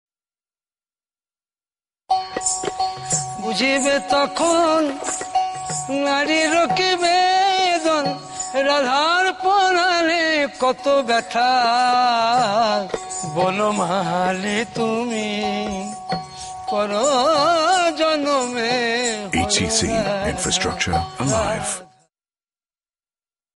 Hindustan Construction Company Advertisement - Idukki Dam
Idukki Dam is an Asymmetrical Parabolic Double Curvature Arch Dam. The project involved the construction of a Concrete Arch Dam across river Periyar at Idukki for Idukki Hydroelectric Project (390 MW capacity), Kerala. The scope of our work included the construction of two dams - the dam at Idukki across river Periyar and the dam at Cheruthoni. The length of the dam is 365.85m.